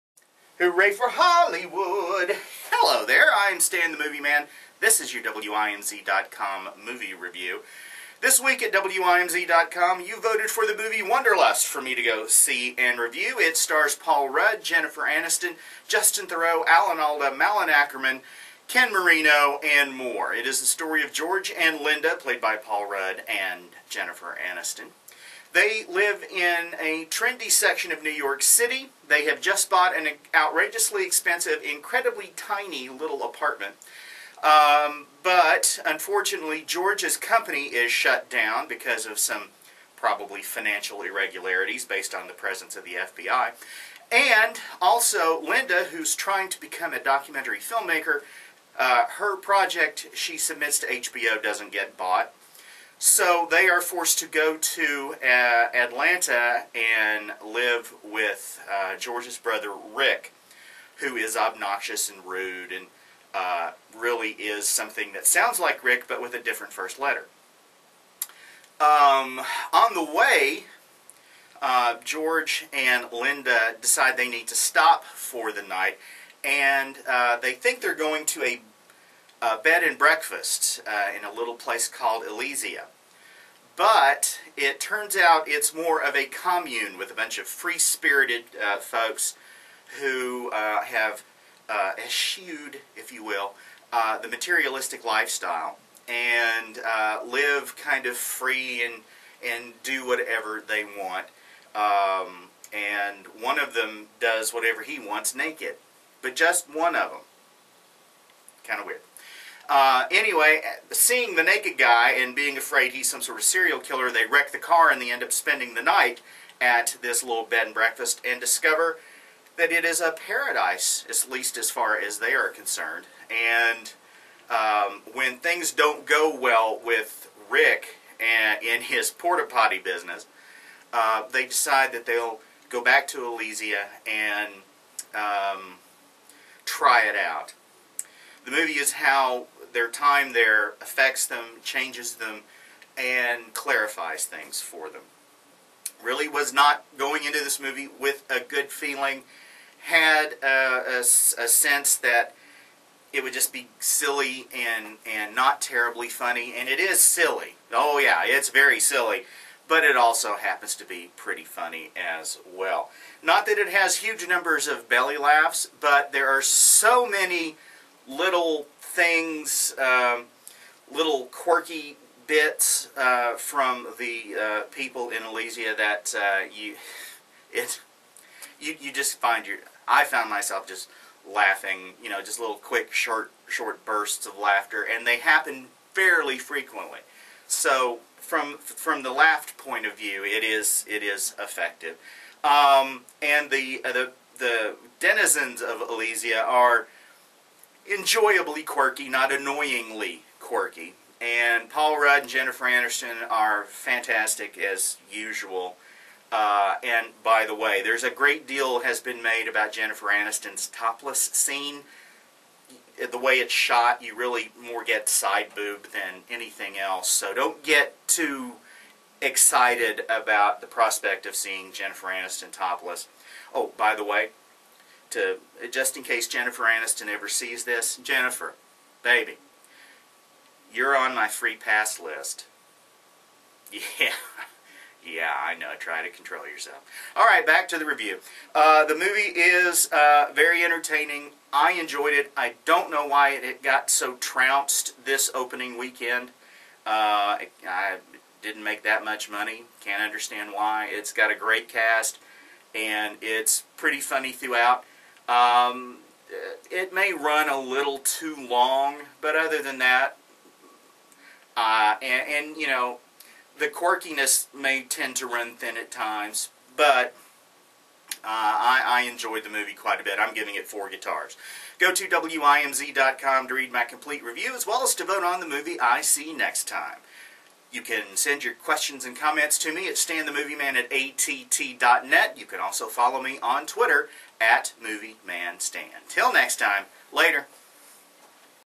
Review of "Wanderlust"
Can a commune full of counter-culture proponents in Georgia change the lives of two uptight New Yorkers needing a fresh start?  Stan the Movie Man from WIMZ.com didn't care but found out anyway in "Wanderlust" starring Jennifer Aniston, Paul Rudd, Justin Theroux, Alan Alda, Malin Akerman and Joe Lo Truglio